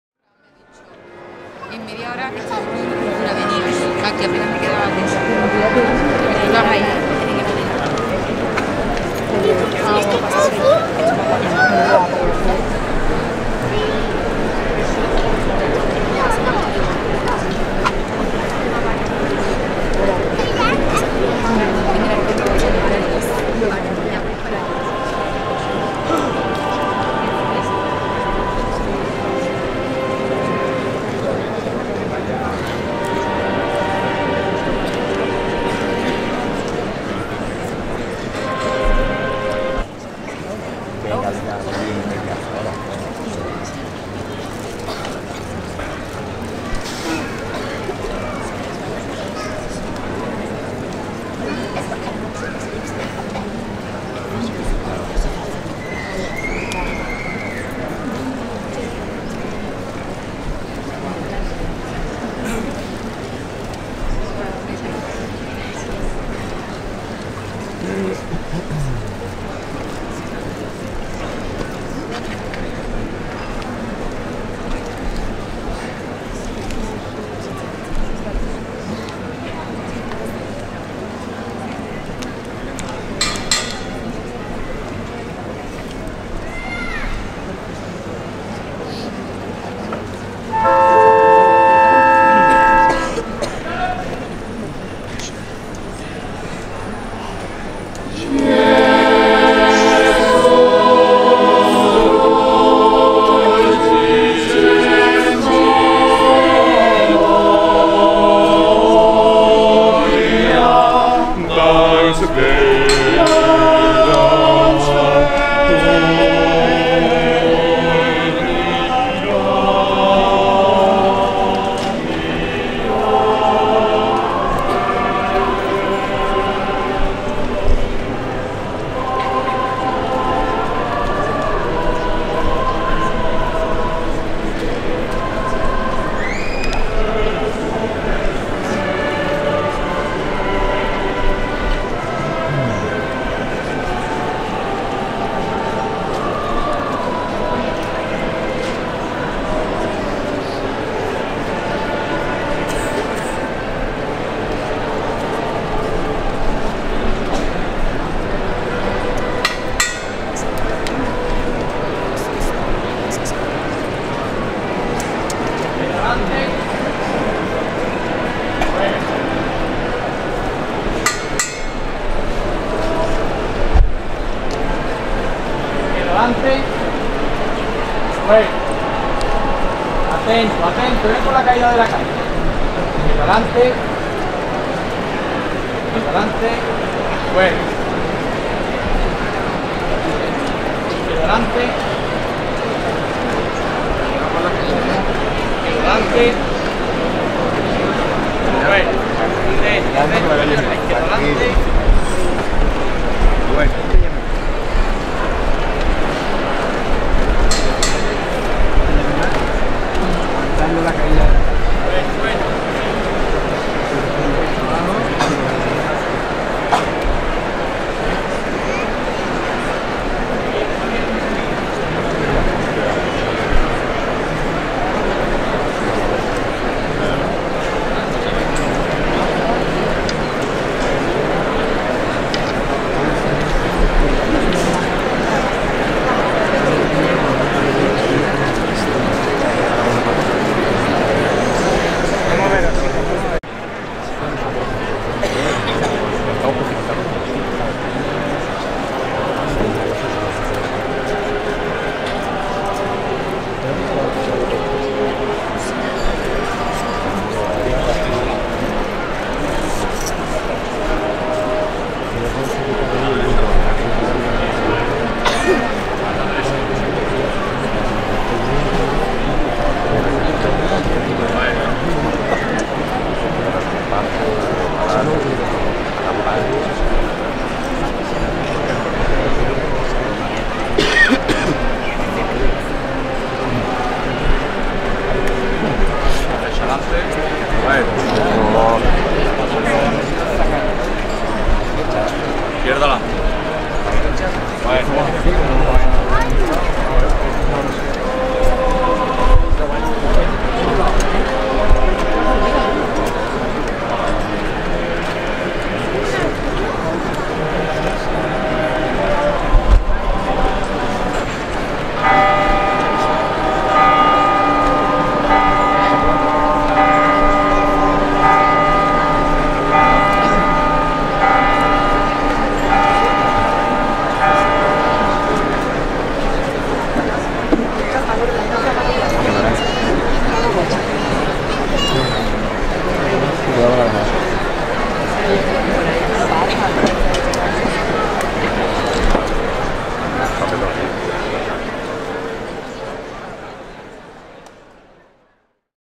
(4K) CRISTO DE LA CORONA en la calle Entre Carceles 2024 |Viernes de Dolore
El Santisimo Cristo de la Corona a su paso por la calle Entre Carceles en la tarde del Viernes de Dolores de Sevilla 2024
Acompañamiento Musical:
Capilla Musical Maria Auxiliadora

The Santisimo Cristo de la Corona as it passes through Entre Carceles street on the afternoon of Friday of Sorrows in Seville 2024
Musical Accompaniment:
Maria Auxiliadora Musical Chapel